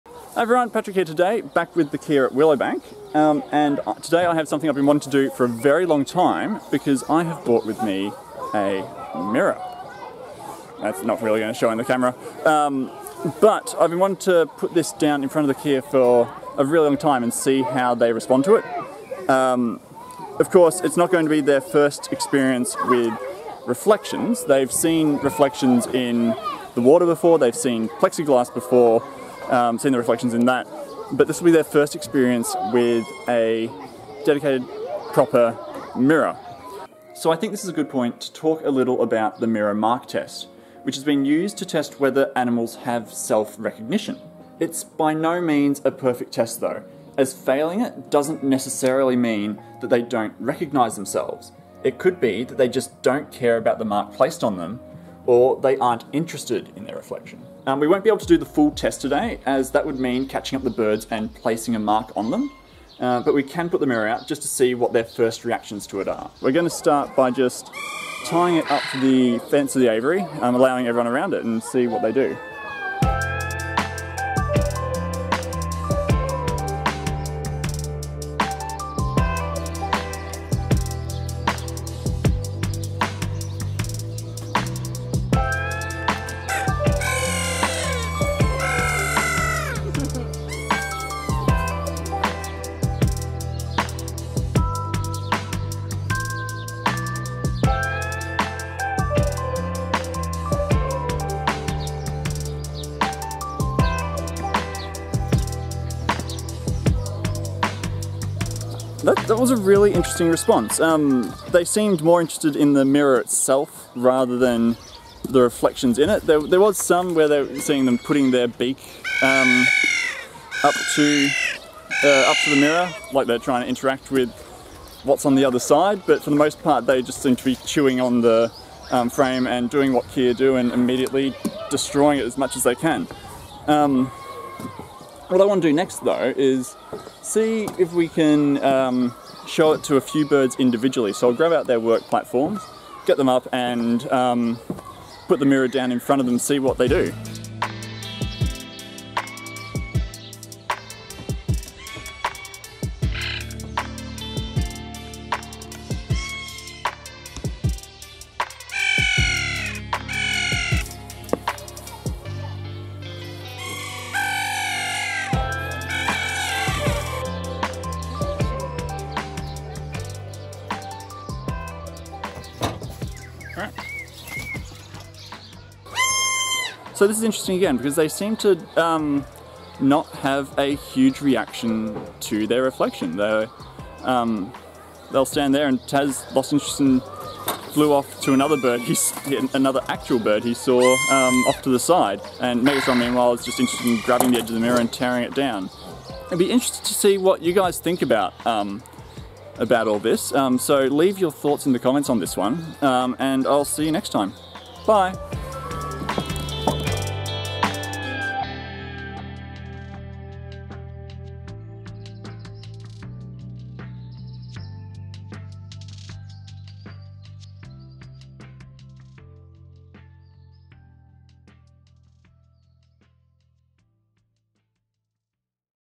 What Happens When You Show Kea A Mirror?
This week we have something I have been wanting to try out for a very long time. I bought in a mirror and put it in front of the kea just to see what their first reaction would be. This could be extended into a proper mirror mark test to see if they are able to recognize themselves at all, but that is beyond what we currently have time or approval to do, so may have to wait for someone else to try in the future!

Consider making a donation to help protect kea in the wild!

Come and visit the kea!